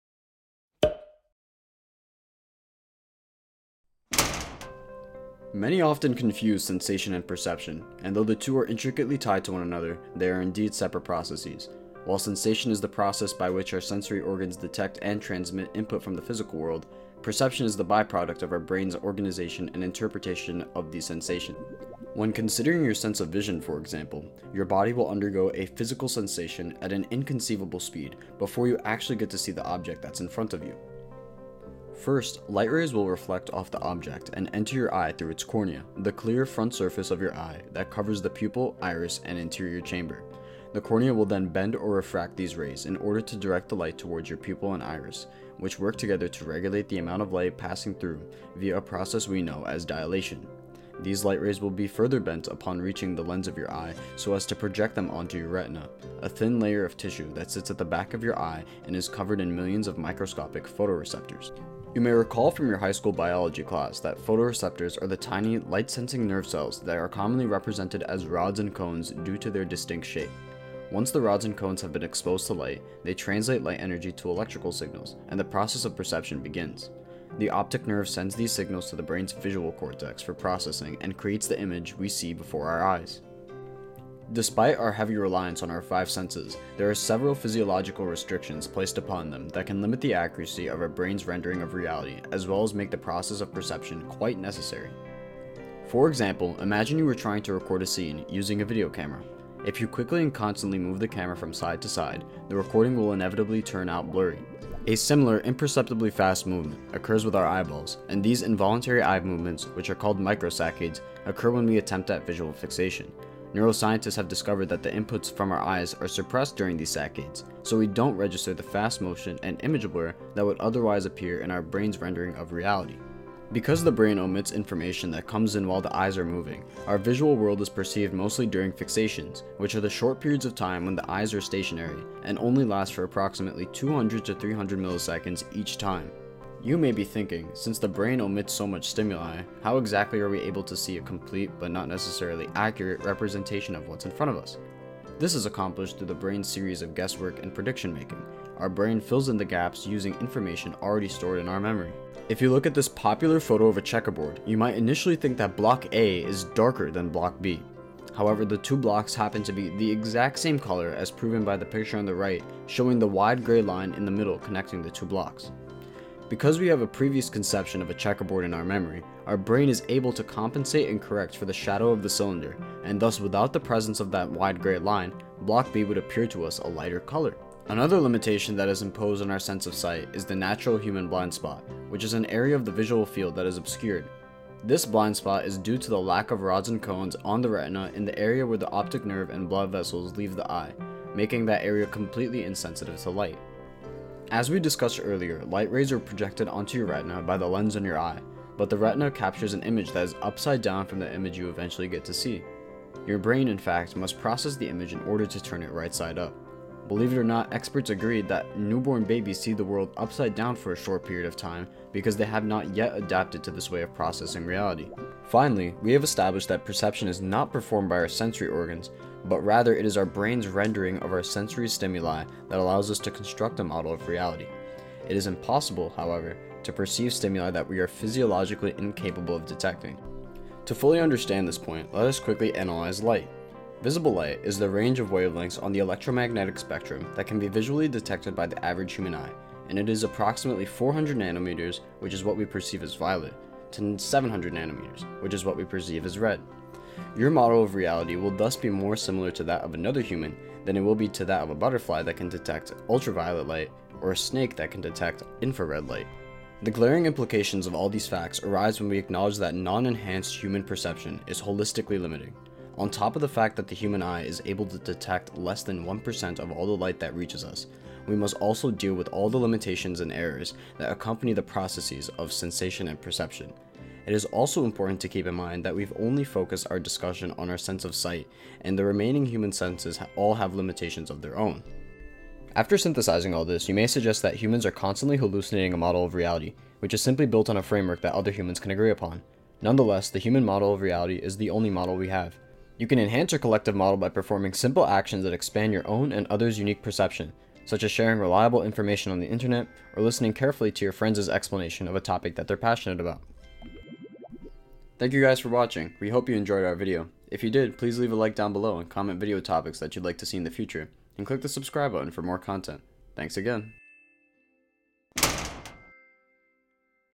Can We Trust Our Perception? — The Illusion of Reality
For our first ever video, we explored the topic of perception and how it allows us to construct our human model of reality. In discussing the inherent flaws of visual perception, we lay down a crucial idea that is fundamental to our coming videos: that everything can and should be questioned. Acknowledging the flaws of human perception promotes free thinking and prevents us from stating any idea as an immutable truth.